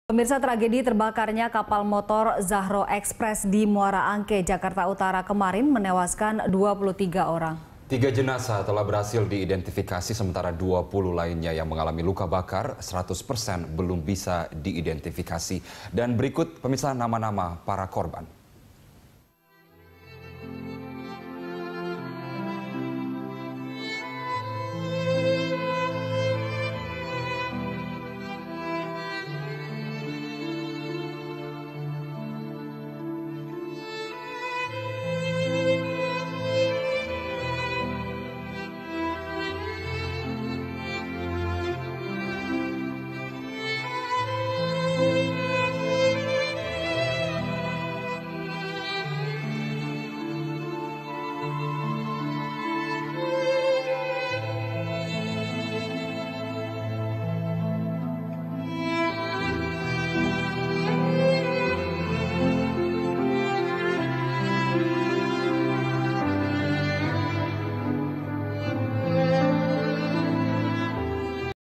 Daftar Korban Tragedi Zahro Express
Tragedi terbakarnya Kapal Zahro Express di Muara Angke kemarin menewaskan 23 orang. Tiga jenazah telah berhasil diidentifikasi. Sementara 20 lainnya yang mengalami luka bakar 100 persen belum bisa diidentifikasi. Berikut nama-nama para korban.

Facebook.com/BeritaSatuTV
Youtube.com/BeritaSatu